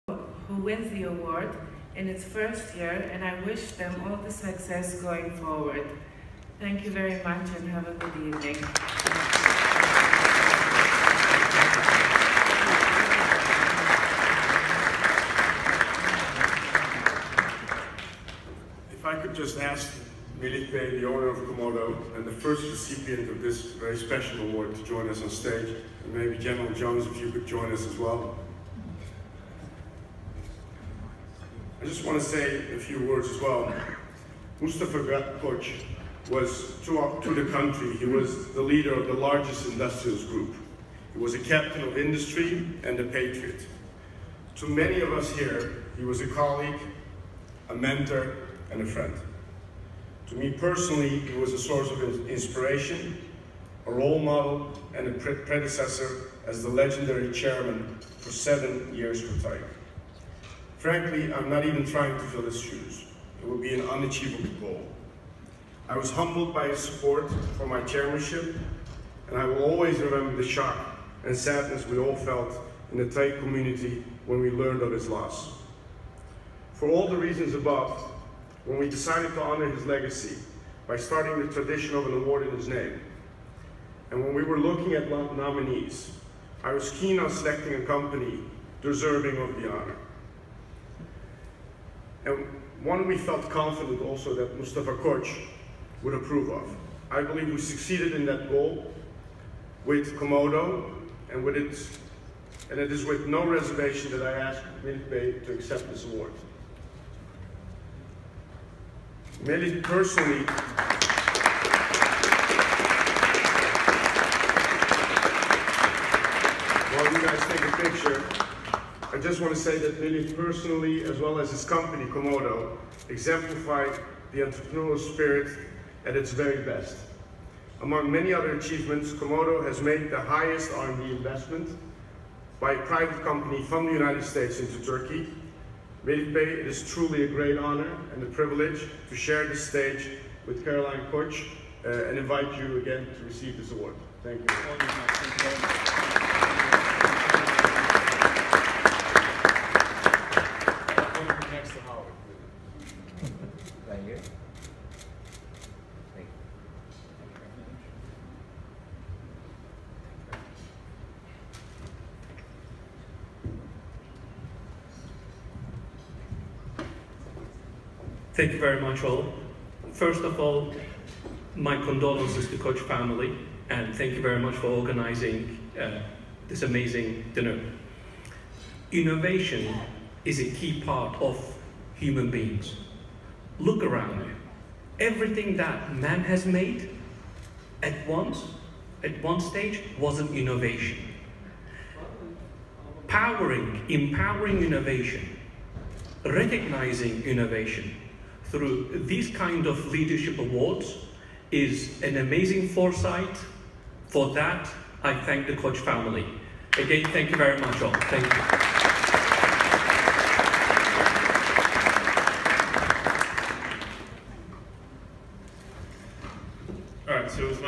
Melih Abdulhayoglu Recieves Mustafa V. Koç Business Leadership Award
35th Annual Conference on U.S.-Turkey Relations to Honor Melih Abdulhayoglu, CEO and Founder of Global Cybersecurity Innovator Comodo
TheAmerican-Turkish Council (ATC) and theTurkish US Business Council are pleased to announce that Melih Abdulhayoglu, CEO and founder of Comodo, a global cybersecurity innovator, will be honored with the “Mustafa V. Koç Business Leadership” Award at the35th Annual Conference on U.S.-Turkey Relations.  Abdulhayoglu is being recognized for making the largest American R&D investment into Turkey.

“Comodo’s ultimate goal is to create trust online. We are making safety in cyberspace achievable by providing consumers and businesses across the globe with the right solutions and strategies, backed by our unique Default Deny Platform. This entirely new approach provides an unprecedented level of security against known and unknown malware by allowing the known good files, blocking the known bad, and then isolating the unknown in automatic containment,” said Abdulhayoglu. “We have always believed in the Turkish economy and the creativity of our Turkish engineers. I can proudly say that we have introduced our innovative products engineered locally into the world market and are growing our investments every day. The creation of our cutting-edge solutions for the cybersecurity market are consistently made possible by R&D investments.”He continued, “Receiving this award bearing Mustafa Koç’s name is truly a monumental honor. Koç was an unbelievable asset to the Turkish economy and business world. We will always remember him for his contributions and innovative approach to enhancing the R&D culture in my homeland. He has inspired Comodo to make substantial contributions to Turkish-American relations, which we certainly plan to continue.”

“The 'Mustafa V Koç Business Leadership' is given in honor of a beloved son of Turkey to a true inspiration and business leader, Mustafa V Koç.  Mustafa Koç’s same spirit lives on in Melih Abdulhayoglu’s dedication to global innovation to ensure that the cyber realm is safe and can be trusted for digital transactions in Turkey, America, and the world.” Howard G. Beasey, President & CEO, American-Turkish Council

“Among the Turkish American role models, Comodo stands out as a technology company that provides high value-add services throughout the world and tirelessly invests in R&D in line with the entrepreneurial vision of Mustafa Koç.” Ekim Alptekin, Chairman of TAIK

The U.S.-Turkey Relations conference,USTurkeyConf, is convening more than 500 leaders and visionaries in the private sector, government, and the non-profit space to shape the future of business, innovation, and growth from October 30 until November 1, 2016 in Washington, DC. More than 20 industries are in attendance, represented by CEOs, entrepreneurs, investors, members from the U.S. Congress and Turkish Parliament, and other ministers and cabinet secretaries.

Diverse and innovative conference sessions include such topics as: Turkey’s Business Environment after the Coup, the 2016 U.S. Elections and its Impact on U.S.-Turkey Relations, Defense Procurement and Industry, Assessing the U.S. Market for Turkish Manufacturers and Investors, The Future of Turkey’s Construction Industry, Turkey’s Tourism Industry, NATO Alliance and U.S.-Turkey Strategic Cooperation, Turkey’s Microeconomic Success,  Renewable Energy and Agriculture, Financial Technology Ecosystem, Telemedicine, and many more.